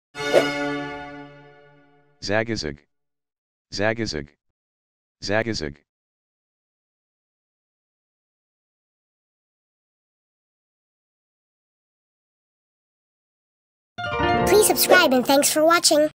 Zagazig (How to Pronounce Cities of the World)💬⭐🌍✅
This video is about "Zagazig". This video series is something special. We're covering the pronunciation of all the cities of the world.